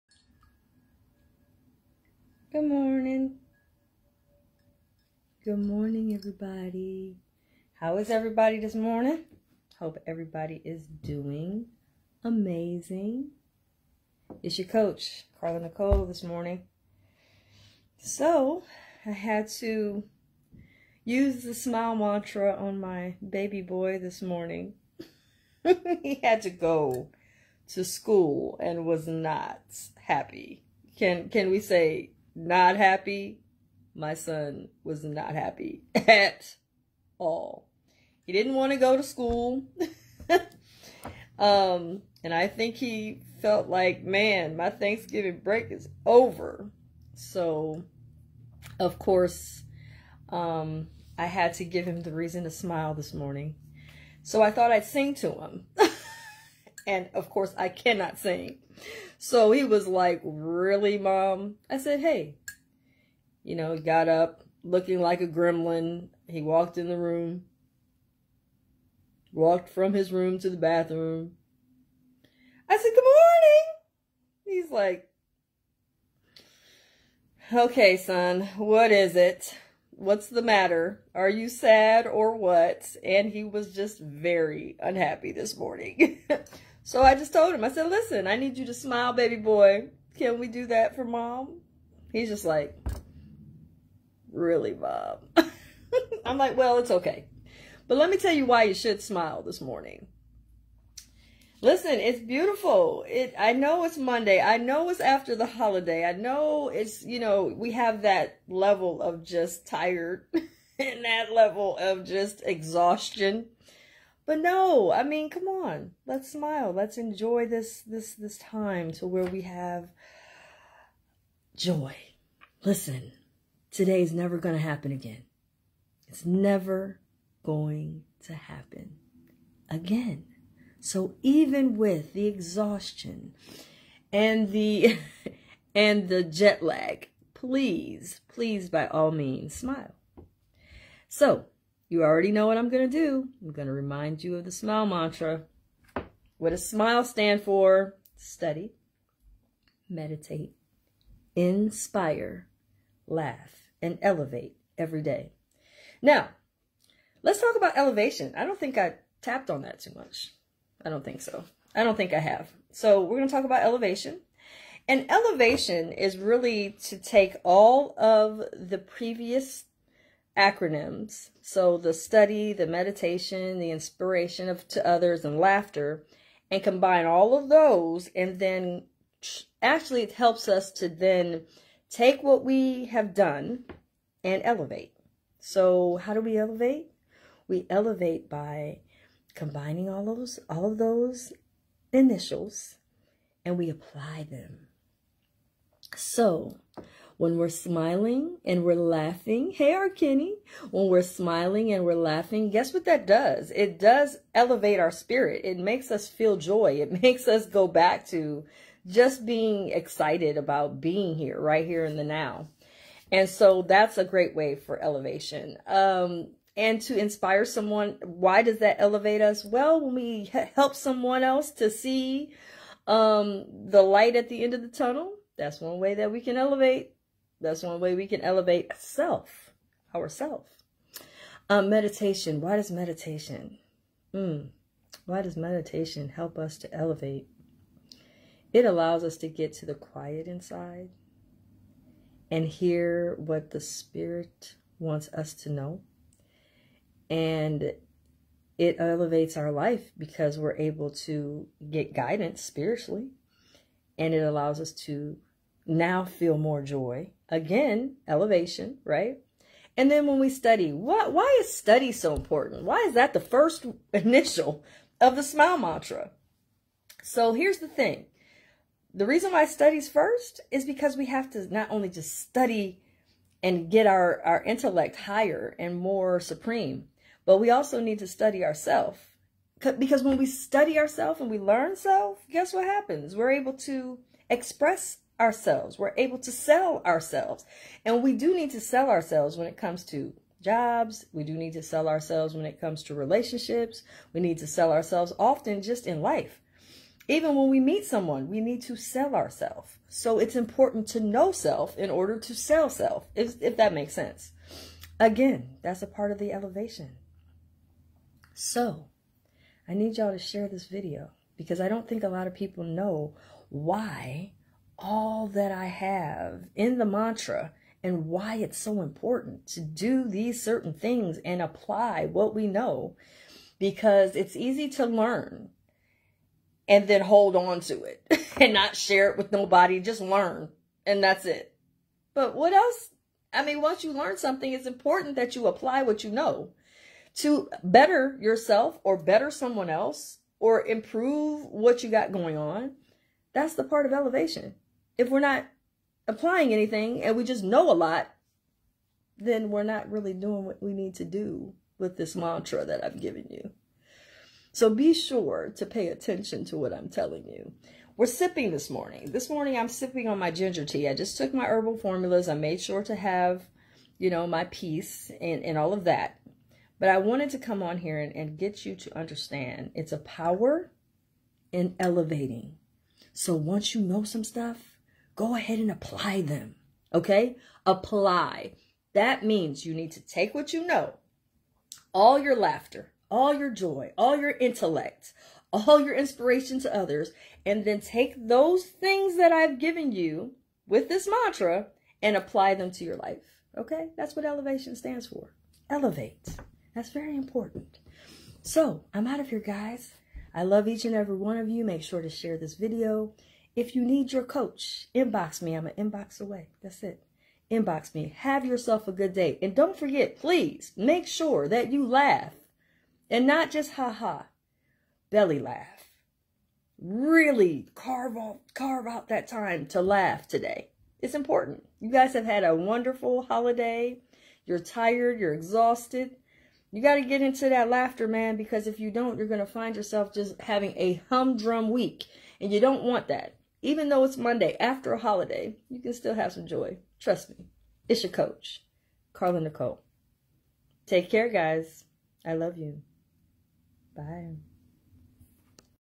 Why Smiling is Good for You! (Self-Care, Self-Love)
My son was very unhappy to attend school on the Monday following his winter break. So I decided to help him with his agitation by doing this.